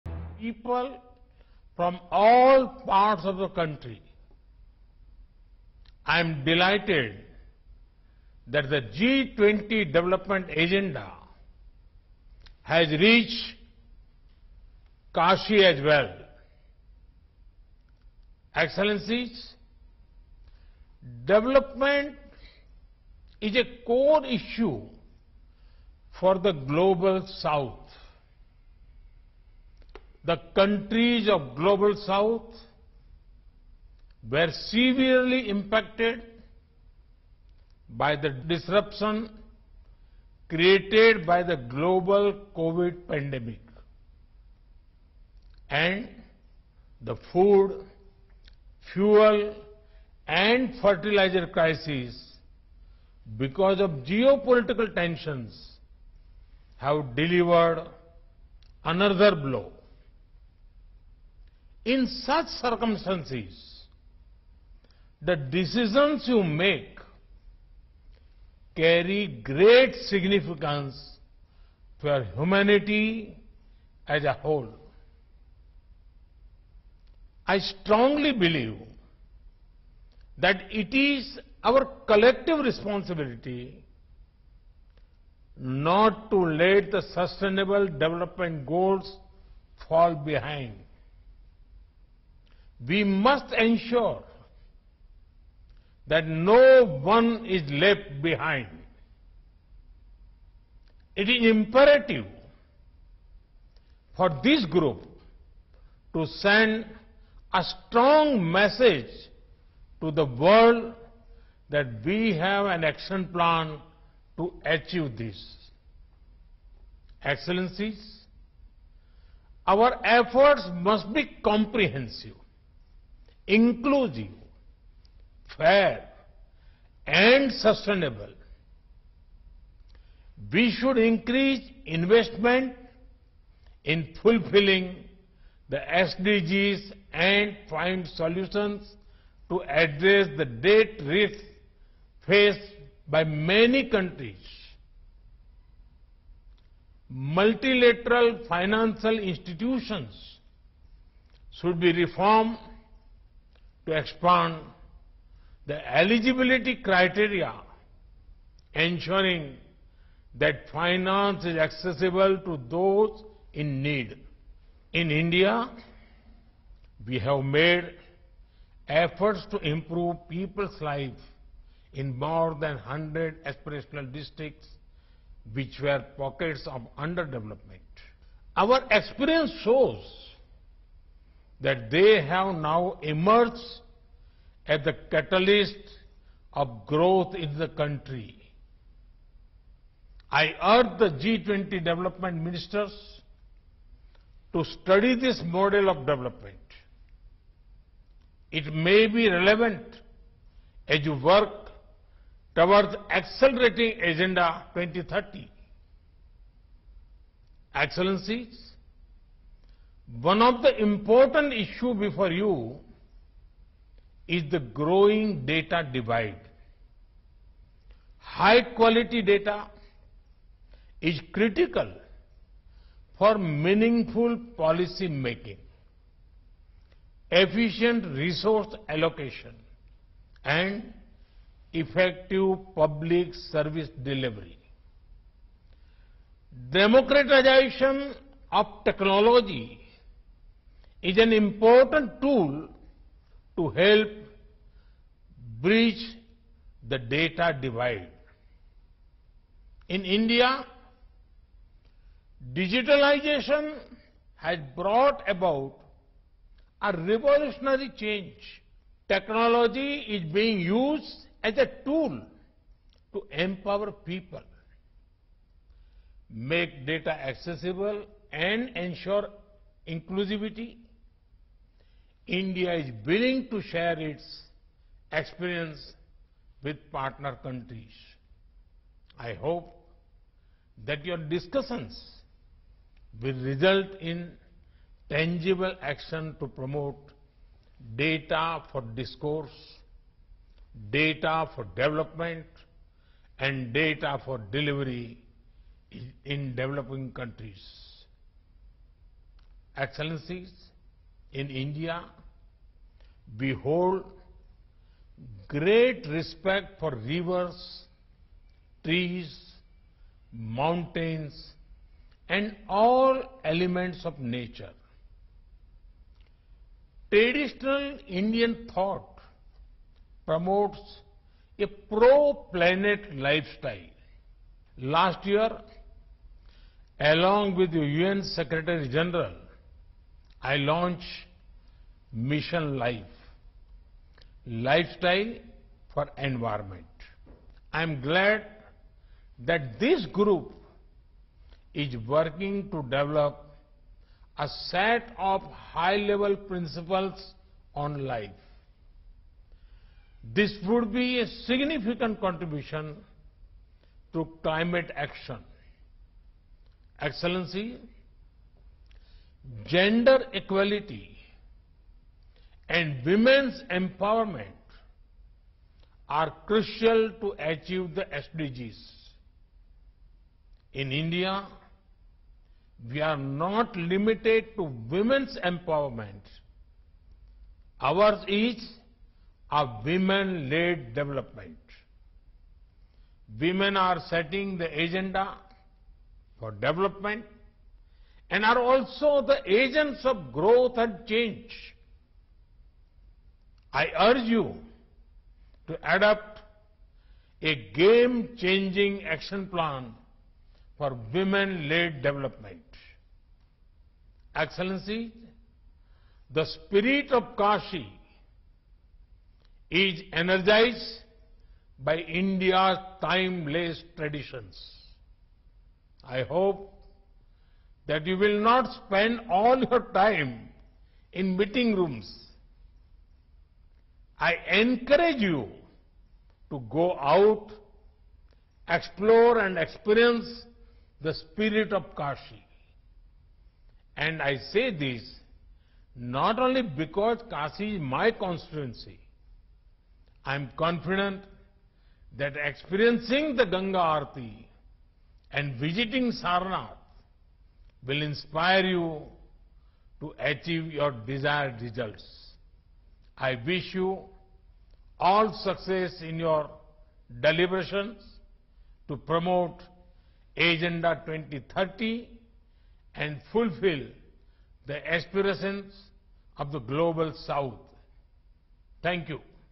PM Narendra Modi Addresses G20 Development Ministers Meet | Watch
The Union Minister of External Affairs Dr S. Jaishankar will chair the G20 Development Ministers’ Meeting which is scheduled to be held between 11-13 June 2023 in Varanasi, Uttar Pradesh. Prime Minister, Shri Narendra Modi will deliver a special video address at the beginning of the meeting, under the G20 Indian Presidency. The Varanasi Development Ministers’ Meeting takes place amidst mounting developmental challenges that have been further aggravated by the economic slowdown, debt distress, impacts of climate change, pollution, and biodiversity loss, growing poverty and inequality, food and energy insecurity, cost-of-living crisis, global supply-chain disruptions, and geo-political conflicts and tensions.